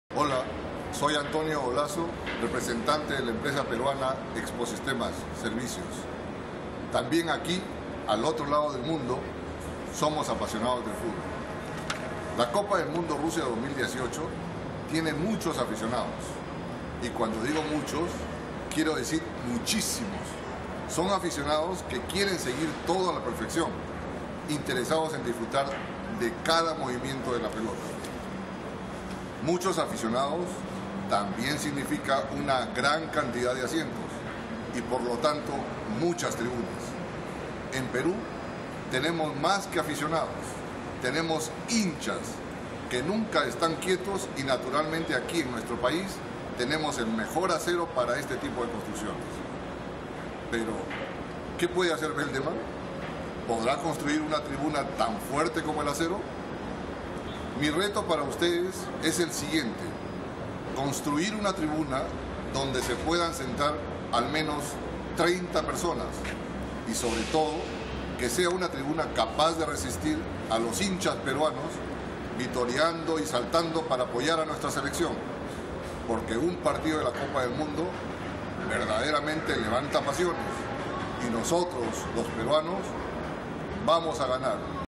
Veldeman for world champion: CHALLENGER PERU
Antonio Golazo from the Peruvian firm Exposistemas is challenging us to create a stand as strong as steel for a minimum of 35 people. It must be one that can withstand cheering and stamping supporters.